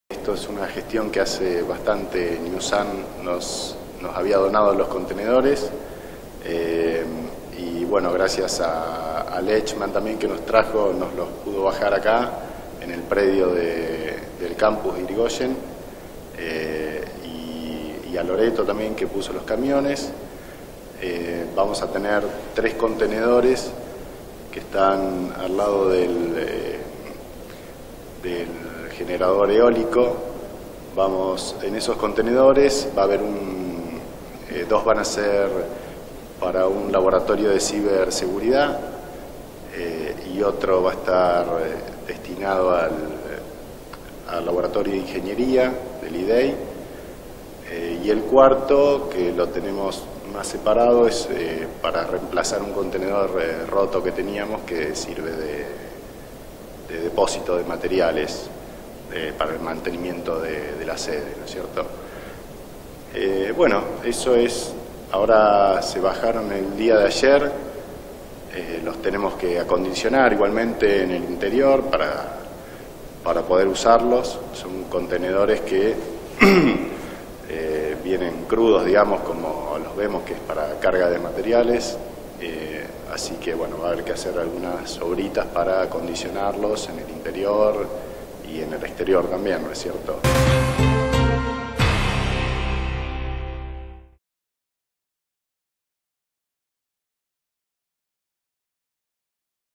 LA EMPRESA NEWSAN ENTREGÓ 4 CONTENEDORES A LA UNTDF.
Con la premisa de ampliar las sedes áulicas.